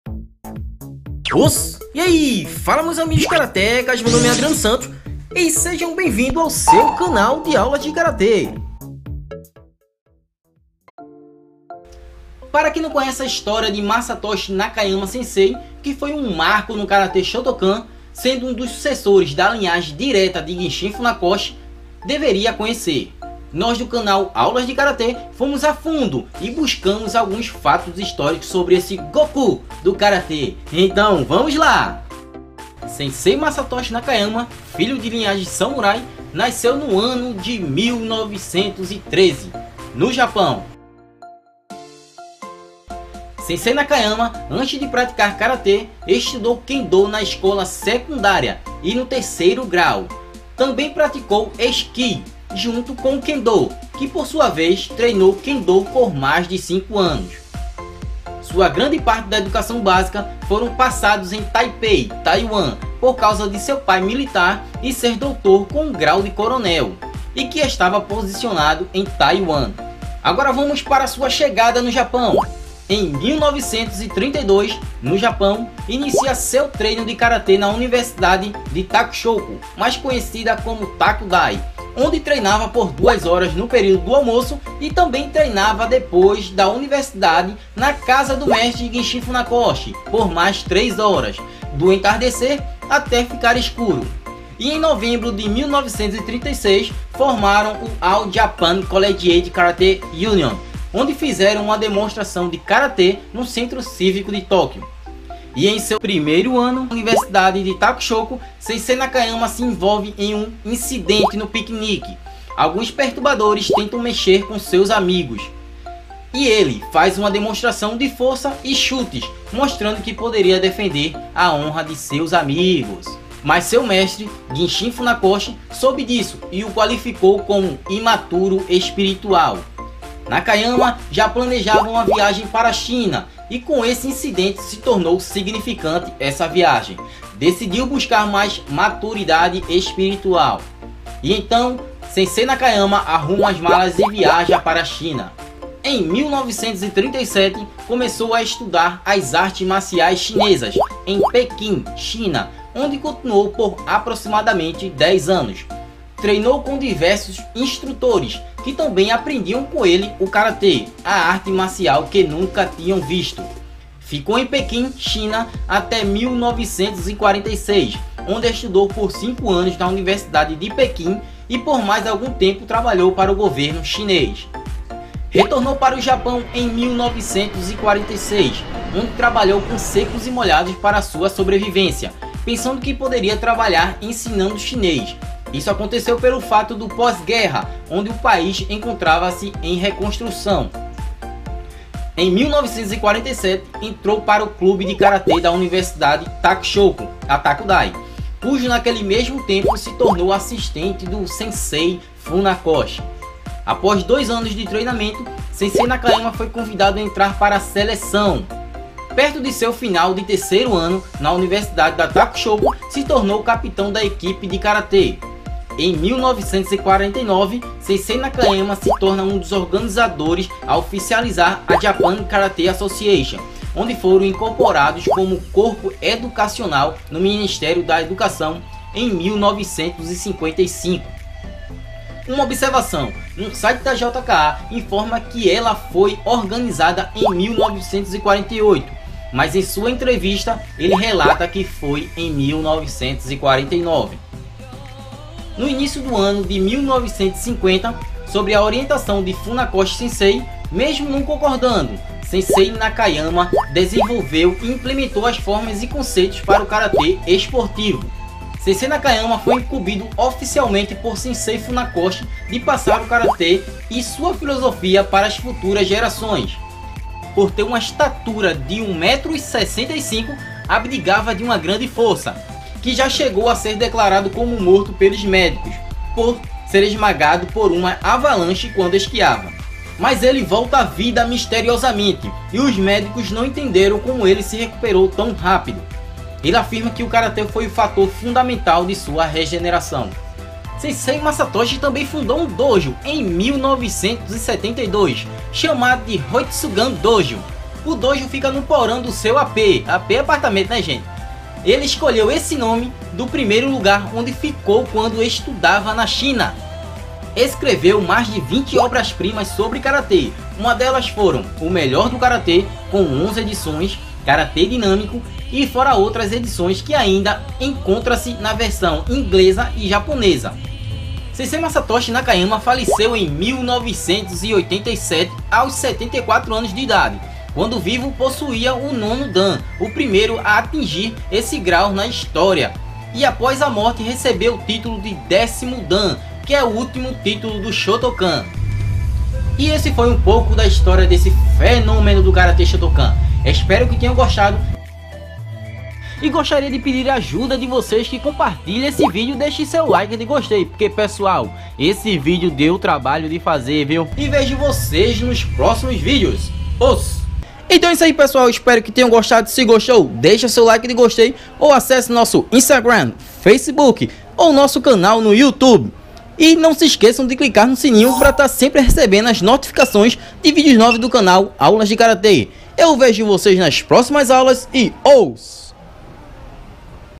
HISTÓRIA DO KARATE - Quem foi MASATOSHI NAKAYAMA
Se você não ver este vídeo é porque não gosta de karate! kkkk